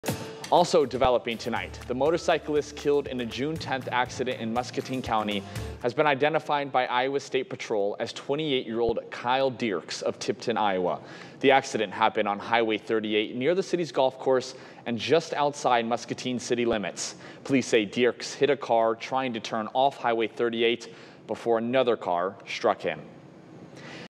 Motorcyclist killed in Muscatine County crash identified
The motorcyclist killed in a June 10 accident in Muscatine County has been identified by ISP as 28-year-old Kyle Dierks of Tipton, Iowa.